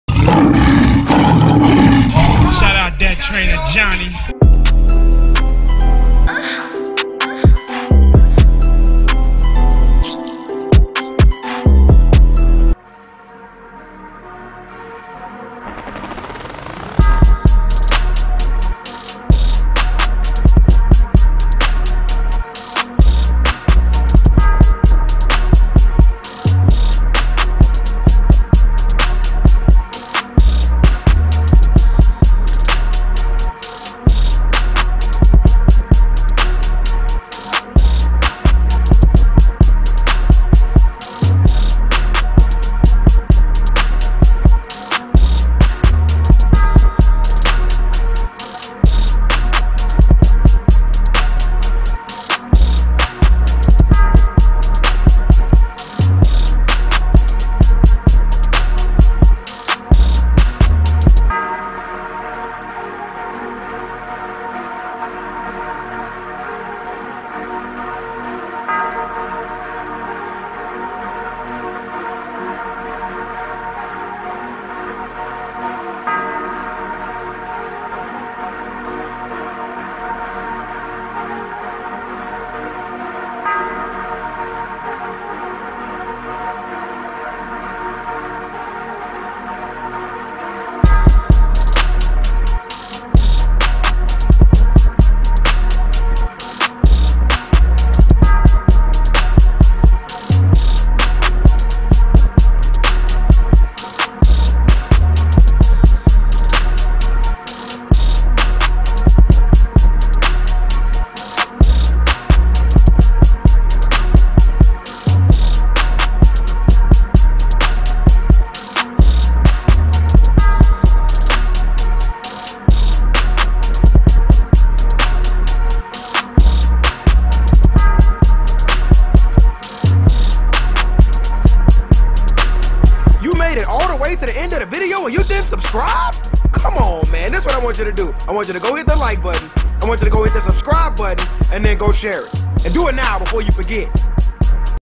THAT TRAINER JOHNNY: Wide Receiver Drills & Training
Check out some footage of a workout I recently had with Dylan Hawk, Sophomore WR at Lutheran HS St. Charles. Easy Work bro!

I post a lot more often on my IG so make sure you hit it up if you wanna see more drills and training techniques @THAT_TRAINER_JOHNNY